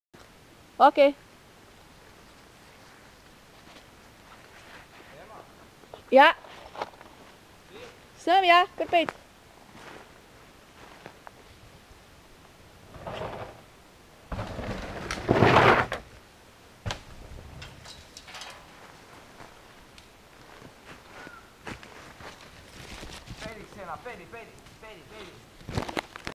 Whip on a wooden kicker
testing my kicker, a day before i cracked my humerus.